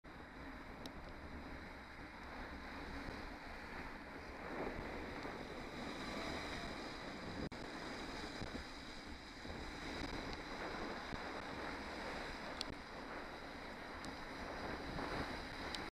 YE08SKV Beechwood Trees and Landscapes Ltd
Pulls out in front of me at a roundabout but since he waved it's acceptable?
Approximately 08:15 Monday 17th March.